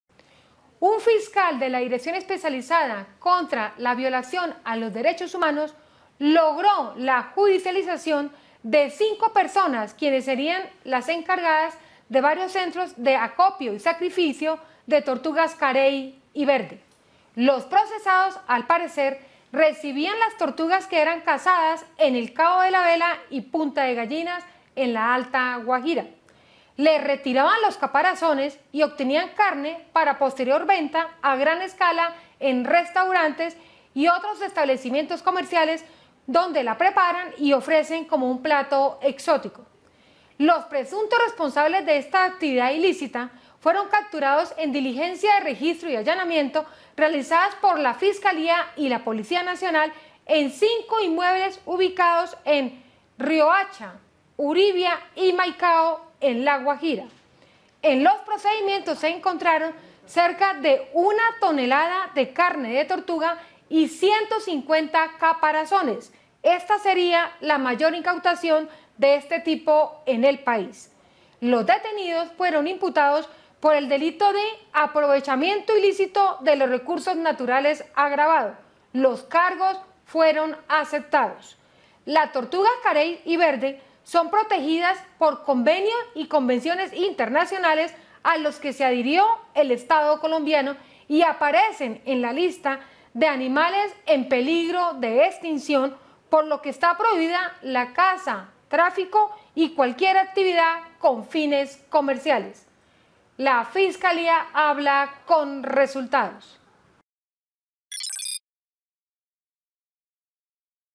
Fiscalía judicializa a presuntos responsables de centros de sacrificio de tortugas carey y verdes
Fiscalía judicializa a presuntos responsables de centros de acopio y de sacrificio de tortugas carey y verdes, dos especies en peligro de extinción. Durante los procedimientos se logró la mayor incautación de carne de tortuga en el país, cerca de una tonelada.

De acuerdo con el material de prueba, los procesados, presuntamente, recibían las tortugas que eran cazadas en las costas del Cabo de la Vela y Punta de Gallinas, en la Alta Guajira; les retiraban los caparazones y obtenían la carne para su posterior venta a gran escala.